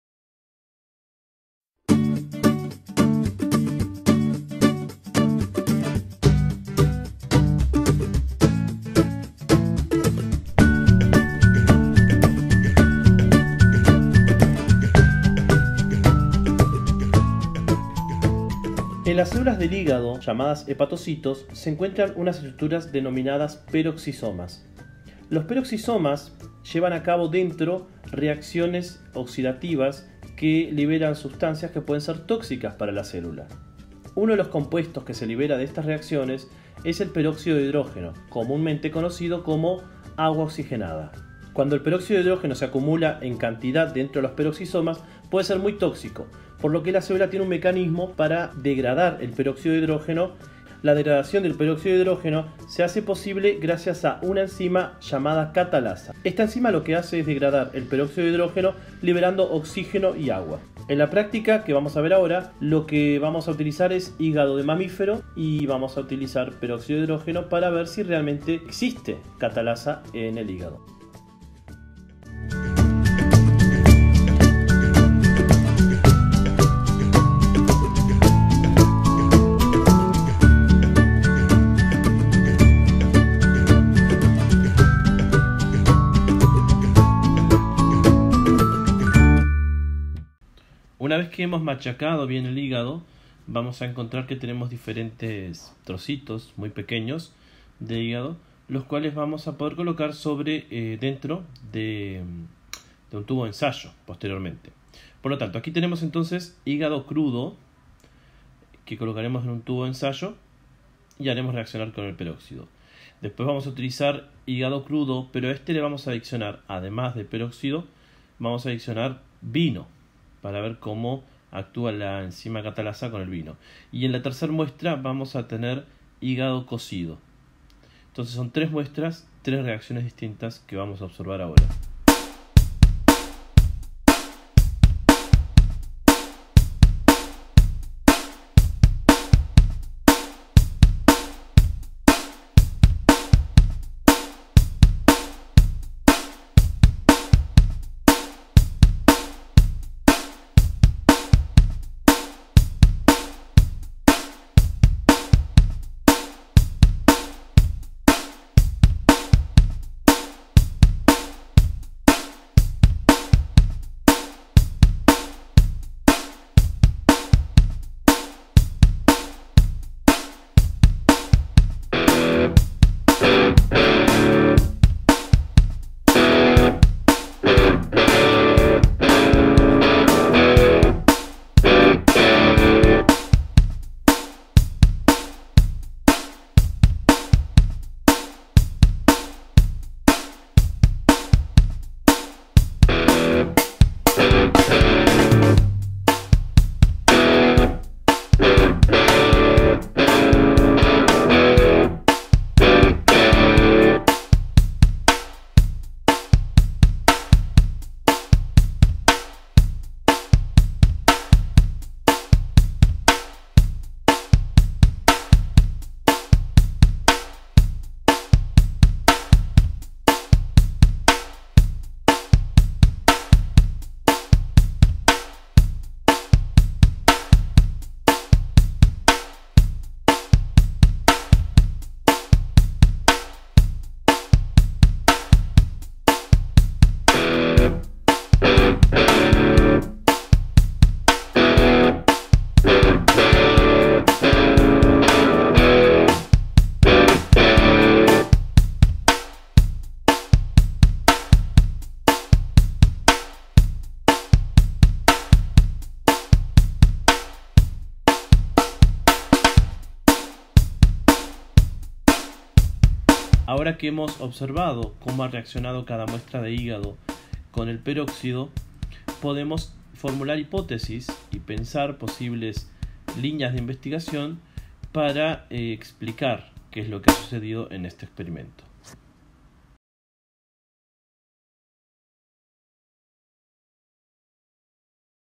Práctica: acción enzimática con modelo de la enzima catalasa.
Trabajo elaborado por los profesores:
Daniel Da Rosa / Sergio Tejera / Daniela Cuesta

Práctica: acción enzimática. Demostración de las actividades y propiedades enzimáticas utilizando el modelo de la enzima catalasa.

Objetivos:
- Demostrar la actividad de la enzima catalasa en tejido animal.
- Observar cómo afecta el vino la actividad de la catalasa.
- Hipotetizar e investigar en base a las observaciones realizadas.